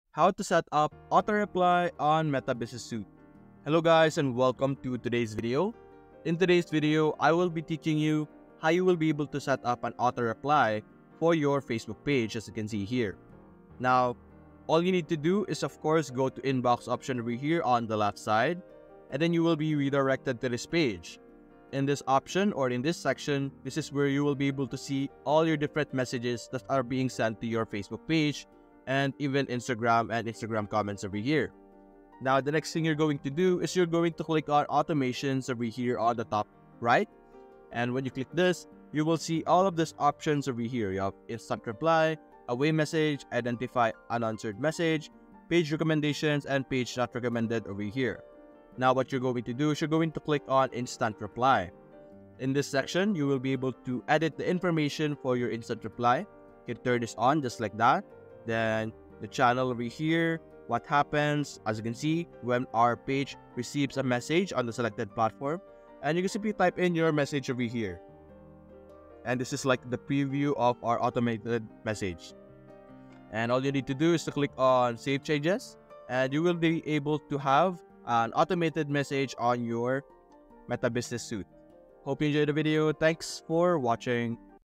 How to Setup Auto Reply in Meta Business suite (FULL GUIDE)
How to Setup Auto Reply in Meta Business suite. In this video, I'm going to show you how to setup auto reply in the Meta Business suite. Auto reply is a great way to keep your email correspondence organized and on track.

By setting up auto reply in the Meta Business suite, you'll be able to handle your email correspondence in a organised and timely manner. This video is a full guide on how to setup auto reply in the Meta Business suite, so be sure to watch it to learn all the details!
How to Setup Auto Reply in Meta Business suite